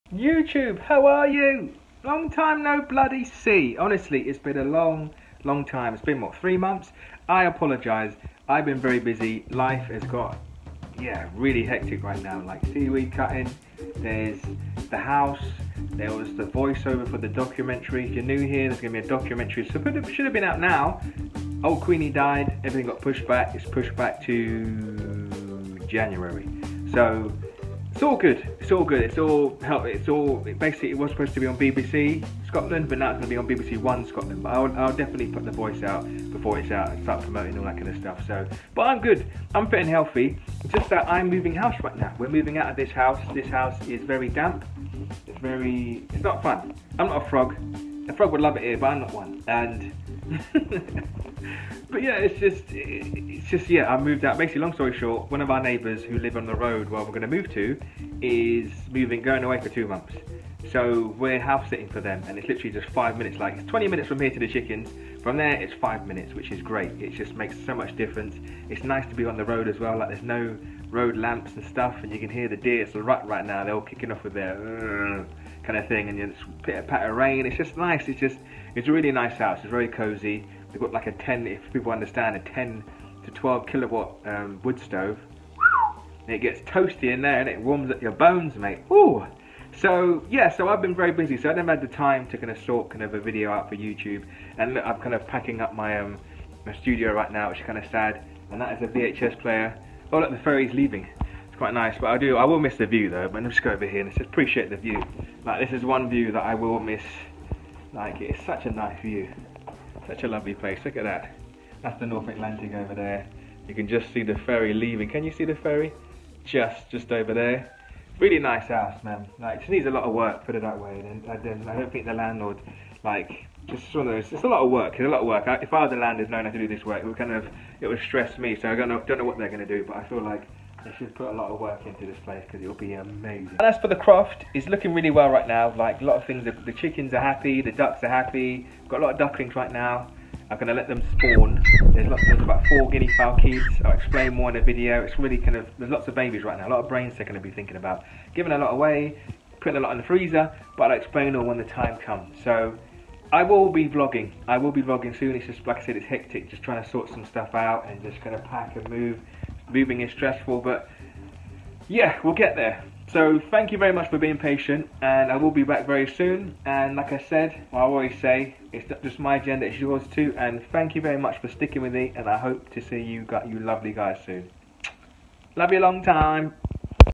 I'M ALIVE/ The WINDIEST permaculture project on Youtube/ the house build and moving house
Hello everyone,
So so sorry for the lack of content. Late summer/autumn is always a busy time for me.
It’s as if I have to have my last bit of fun before the wet and windy days kick in. Plus my work is very weather dependent.
I’ll be back with a tour in the near future so hang tight for that. Love you long time folks x